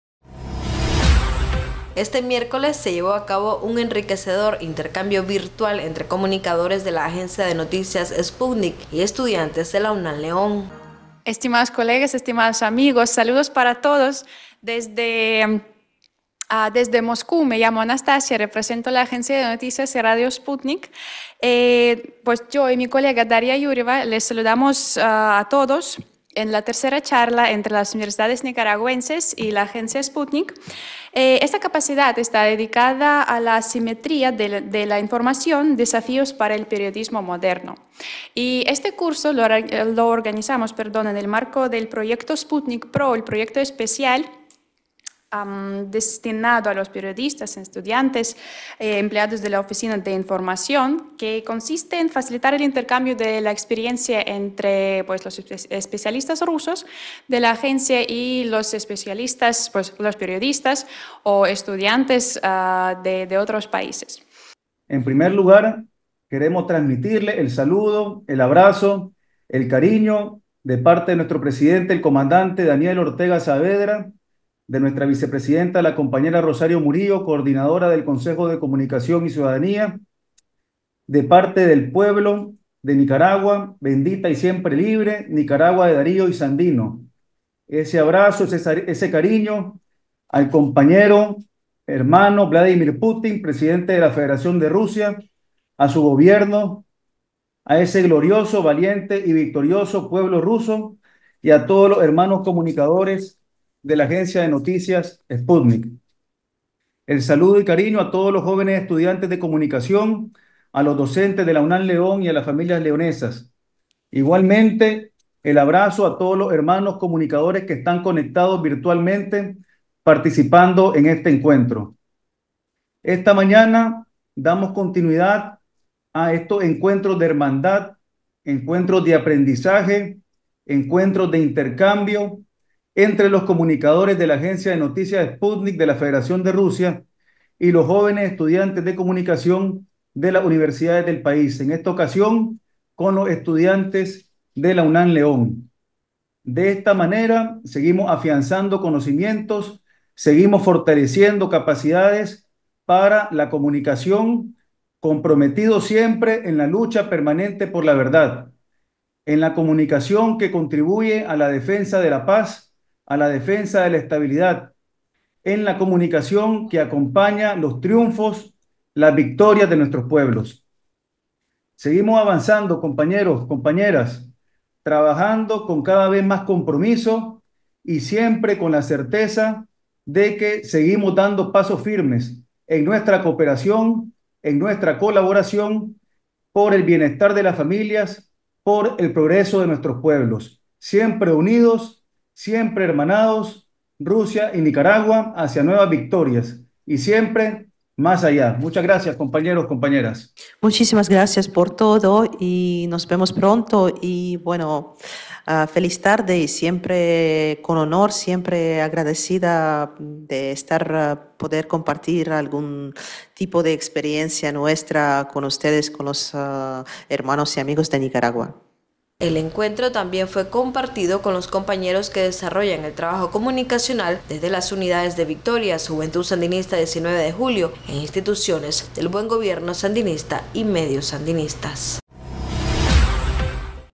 Comunicadores de la Agencia de Noticias Sputnik y estudiantes de la UNAN-León en encuentro virtual
Este miércoles se llevó a cabo un enriquecedor intercambio virtual entre comunicadores de la Agencia de Noticias Sputnik y estudiantes de la Universidad Nacional Autónoma de Nicaragua (UNAN-León). Durante esta colaboración, se discutieron aspectos relacionados con la asimetría en la comunicación y los desafíos que enfrenta el periodismo moderno.

Anastasia Bychkova, representante del Departamento de Cooperación Internacional de la Agencia de Noticias Sputnik, afirmó que esta fue la tercera reunión entre las universidades nicaragüenses y la agencia que ella representa.